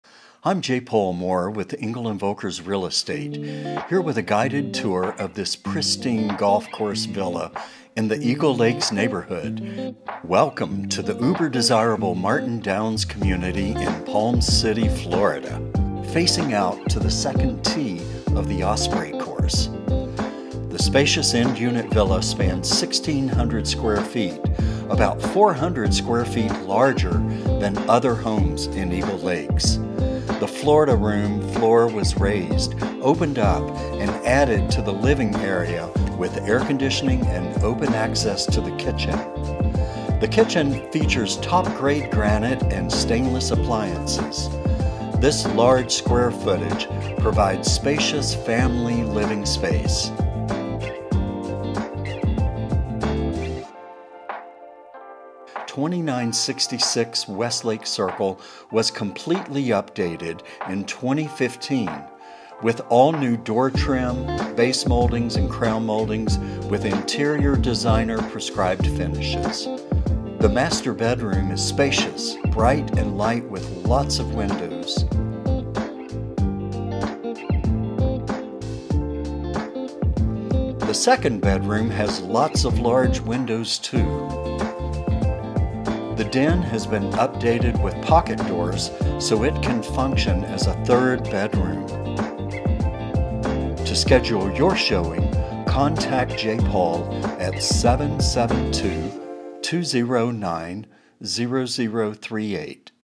Pristine Golf Villa in Palm City, Florida
This golf villa in Palm City's desirable gated & secure Martin Downs community is move-in ready for living a comfortable country club lifestyle! The largest end-unit villas INCLUDES membership at The Village Club & Preserve. Completely renovated in 2015 it has been fastidiously maintained with seasonal use. The updates include interior designer-prescribed color palette for walls & ceilings, crown molding, solid-stone kitchen surfaces, upgraded door trim & finishes. 24 porcelain tile throughout living space & bedrooms. Florida room was raised & added to air-conditioned living space that opens out to the 2nd Tee. The architectural changes already finished for den to be optional 3rd bedroom. Roof & exterior maintained by HOA for affordable insurance cost. Furnishings available separately. Optional Village Club dues are $55/mo for access to pools, tennis & pickleball, basketball, fitness trail, dog park & other recreational features. for club amenities.
HOA is responsible for roof and exterior maintenance.  Includes internet, cable, lawn care, pest control and other benefits.

Priced under $400,000!